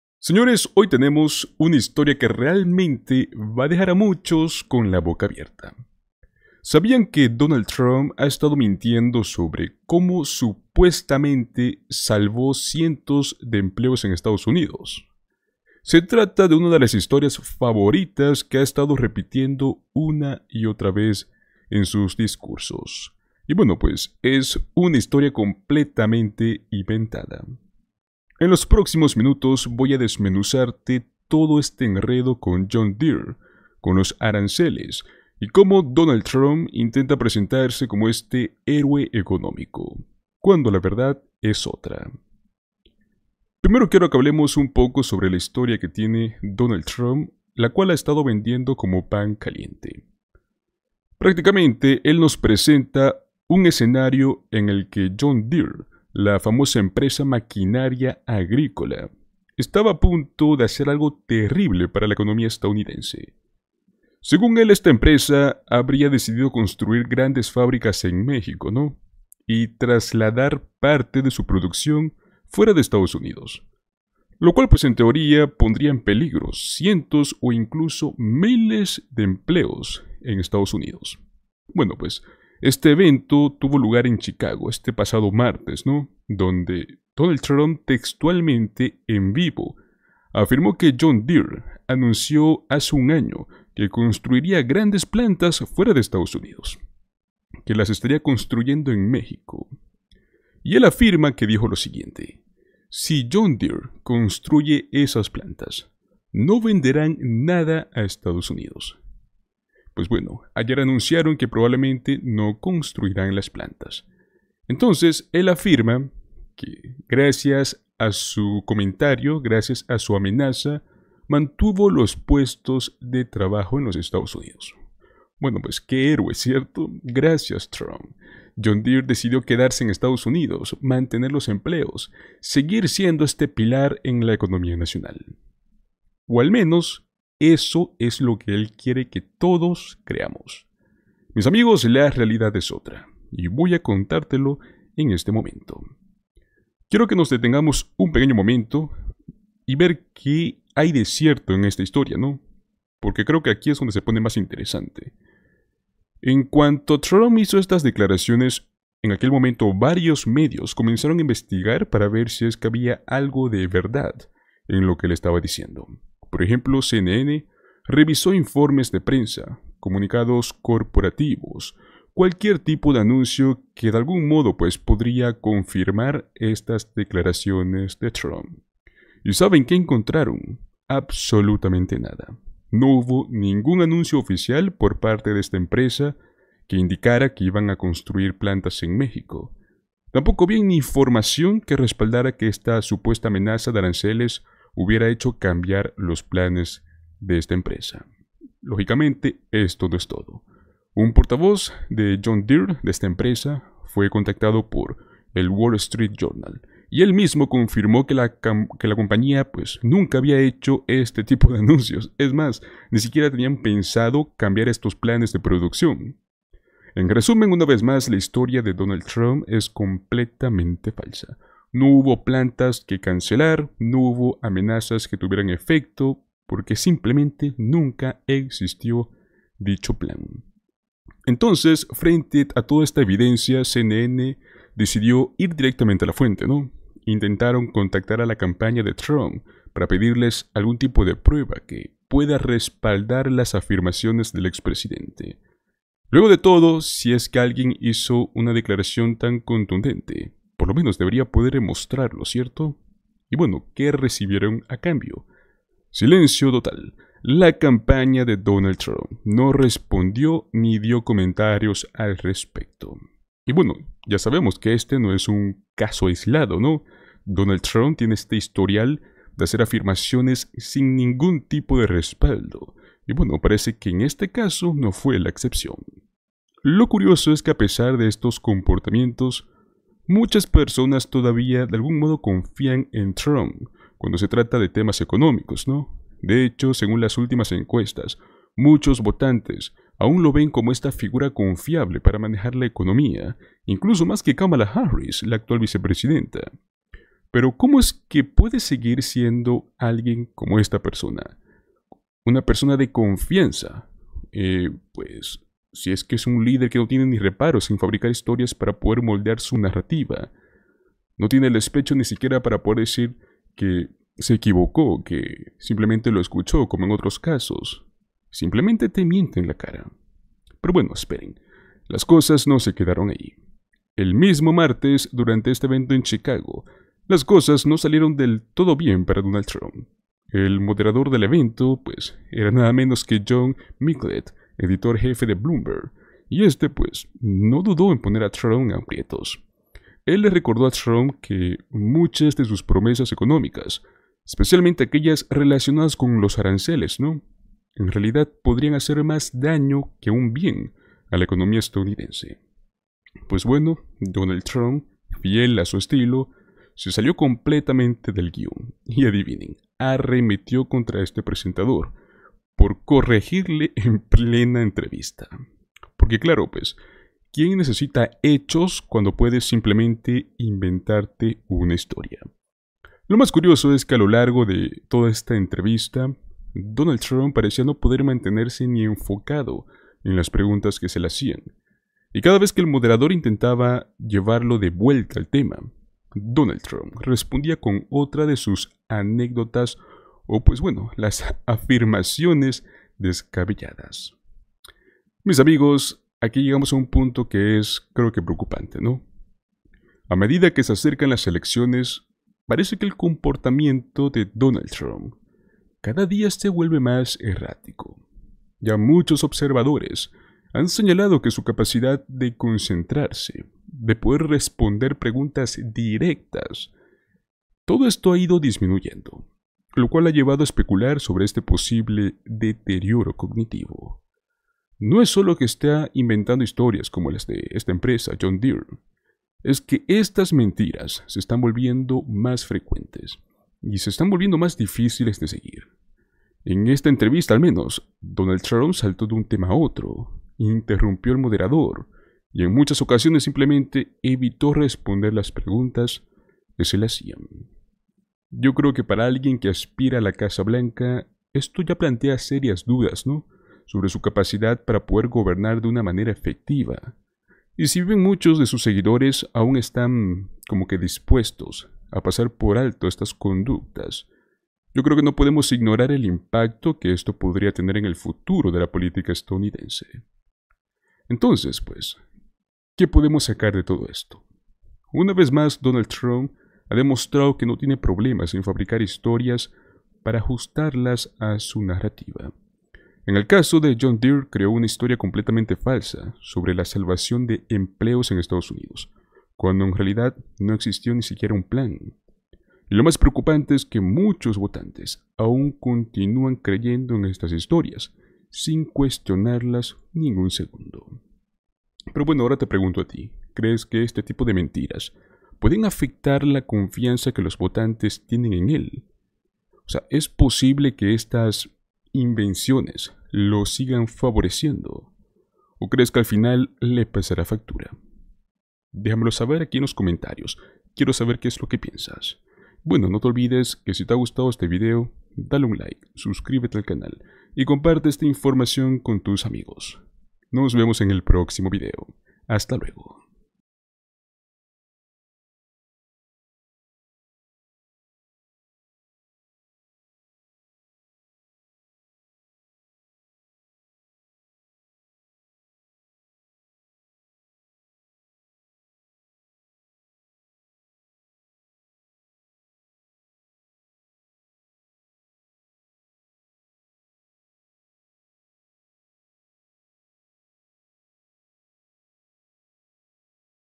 Trump es DESMENTIDO luego de INVENTARSE una Historia Falsa en su Campaña
En un evento en Chicago, Trump afirmó que John Deere, la conocida empresa de maquinaria agrícola, estaba a punto de construir grandes fábricas en México, lo que pondría en riesgo cientos de empleos en Estados Unidos. Dijo: "Si John Deere construye esas plantas, no venderán nada a Estados Unidos. Ayer anunciaron que probablemente no construirán las plantas, ¿de acuerdo? Mantuve los puestos de trabajo aquí." Sin embargo, esta afirmación no tiene fundamento.